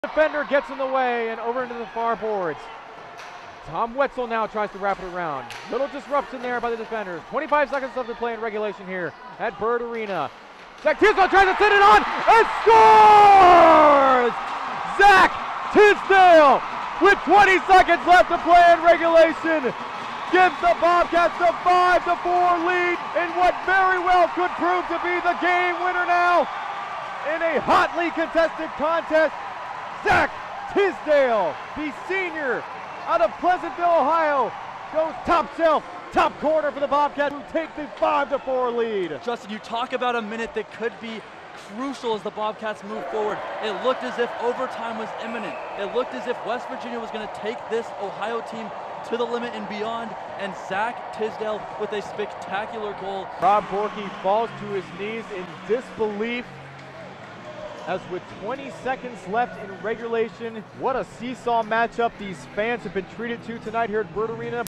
Shot of the Night: Ohio vs West Virginia
Zach Tisdale goes top shelf with 20 seconds left in regulation to put Ohio on top of West Virginia 5-4.